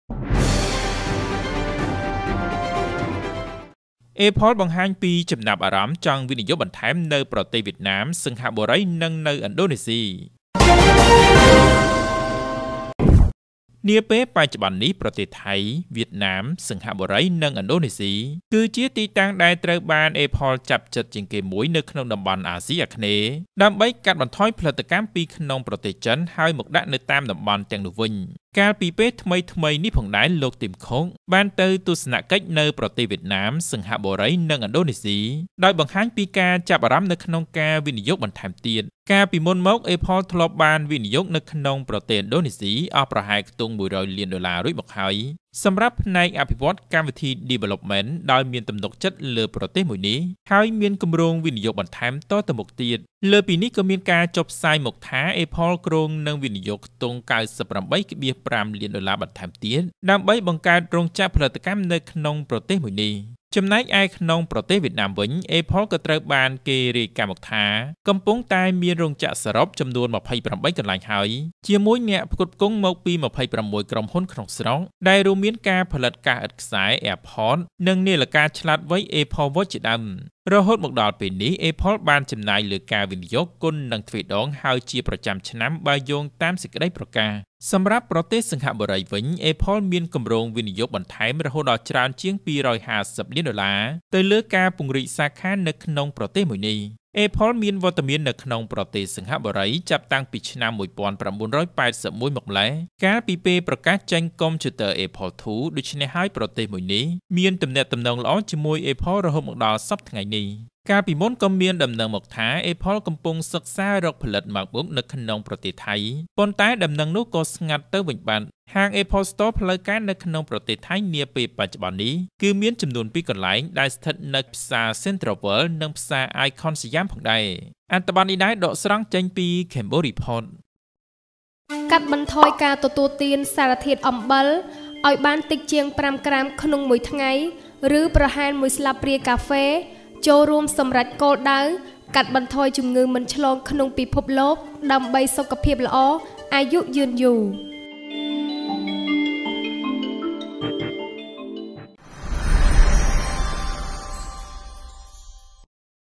Apple បង្ហាញពីចំណាប់អារម្មណ៍ចង់វិនិយោគបន្ថែមនៅប្រទេសវៀតណាម, សិង្ហបុរី និងឥណ្ឌូនេស៊ី
សូមចូលទៅកាន់ FolloW  👇
ព័ត៌មានវីដេអូ៖ ទូរទស្សន៍ជាតិខេត្តត្បូងឃ្មុំ ✅